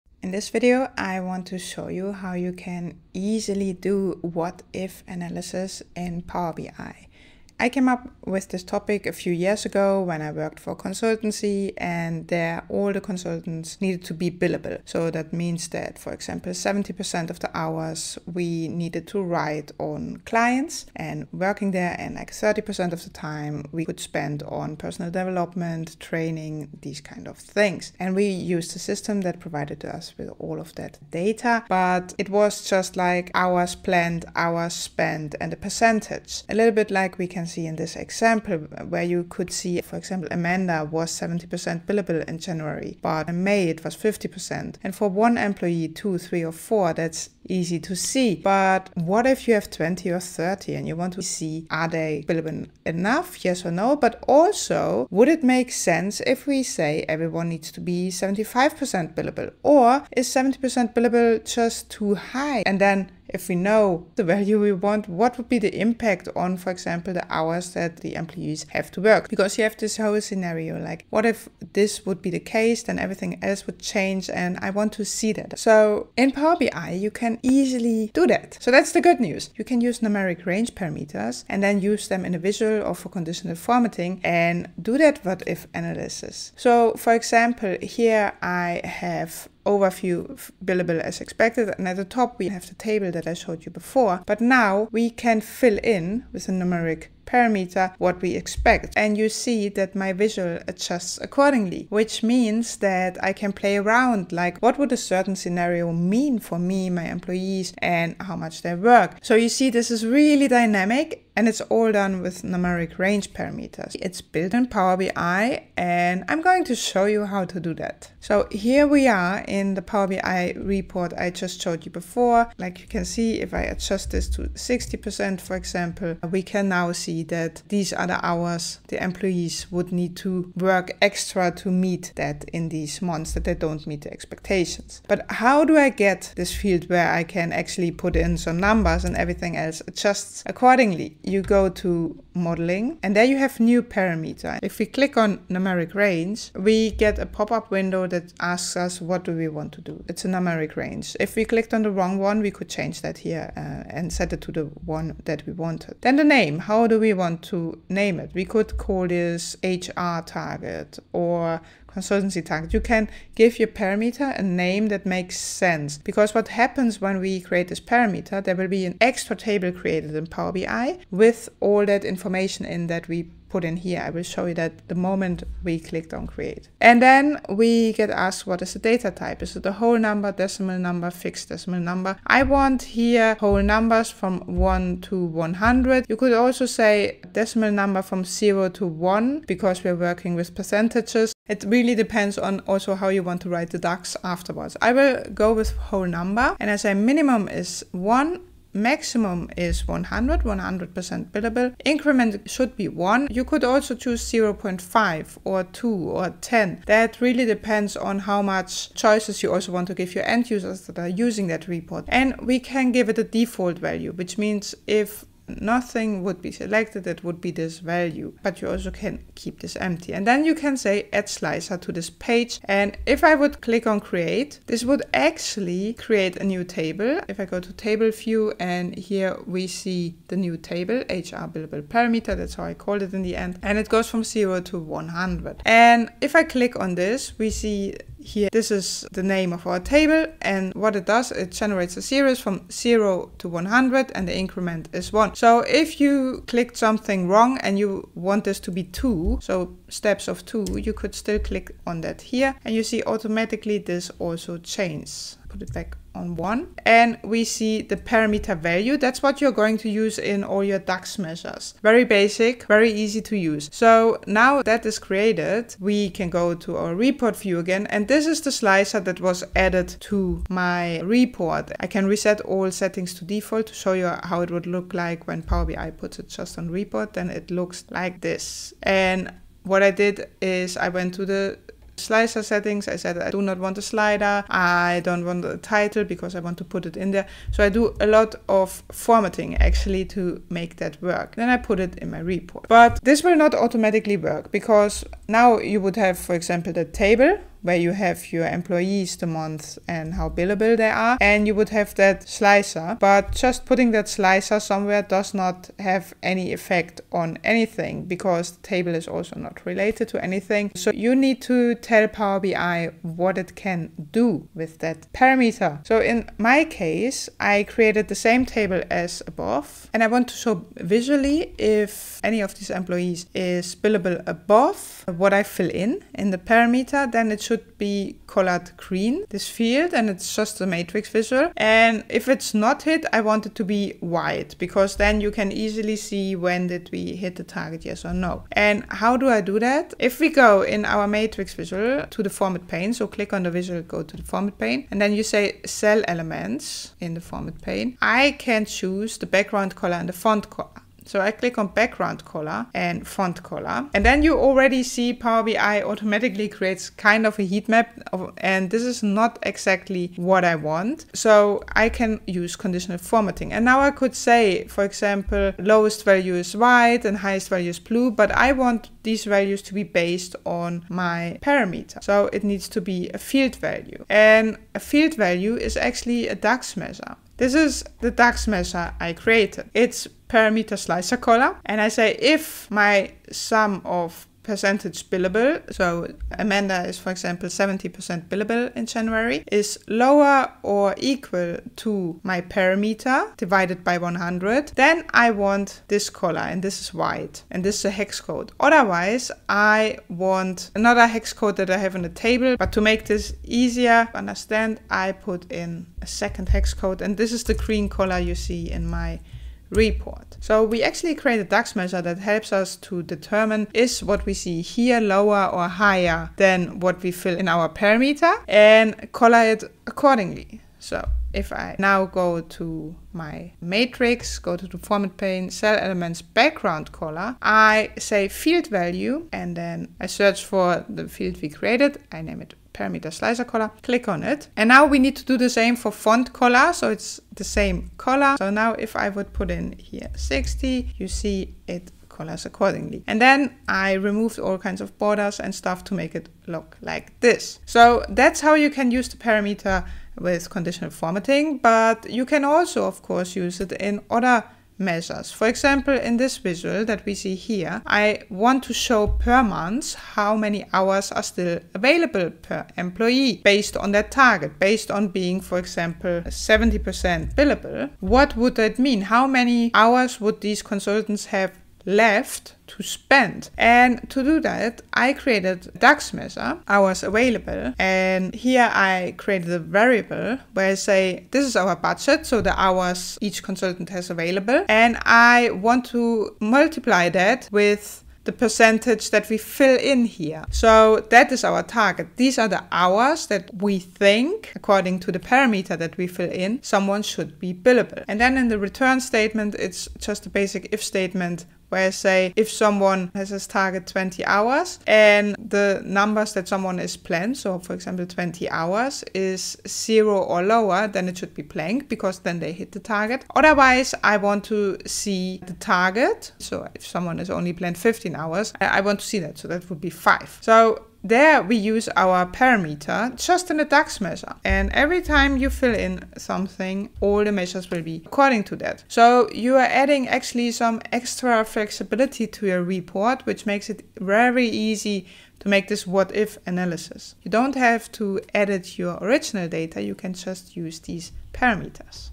What if analysis in Power BI – Numeric Range Parameter
What if your target was 75% billable instead of 70%? Would it still be realistic? That was the kind of question I had back when I worked at a consultancy firm.
In this video, I show how to use numeric range parameters for what-if analysis.
Change the target, see what happens. No need to touch your data!

⏰ TIMESTAMPS
00:00 Intro
02:18 Power BI Demo: What If Analysis
02:22 Configuring numeric range parameter
03:44  New parameter table
04:40 Formatting the parameter slicer
05:14 Using parameters in DAX measures for conditional formatting
07:38 Numeric parameters to create new measures for visuals